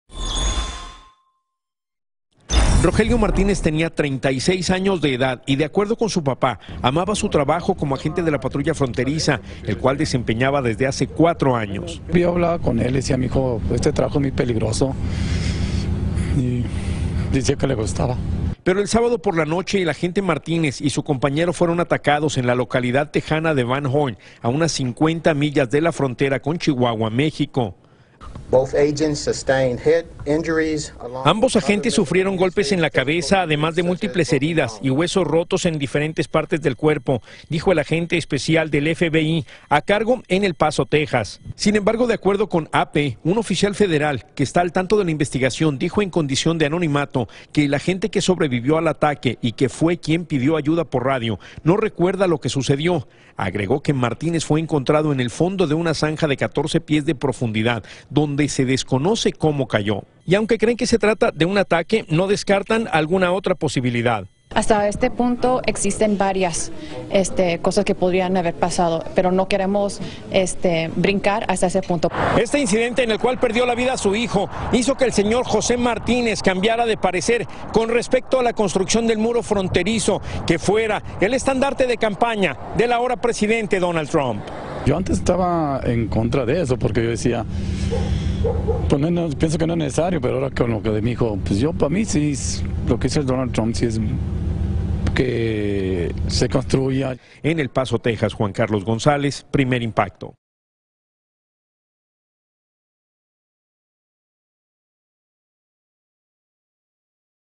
"Le dije que el trabajo era peligroso": Padre del agente hispano que murió en la frontera
El padre de Rogelio Martínez contó que su hijo amaba su trabajo, a pesar de que le advirtió que era peligroso. Dijo que antes no creía en la construcción del muro, pero cambió de opinión tras su muerte.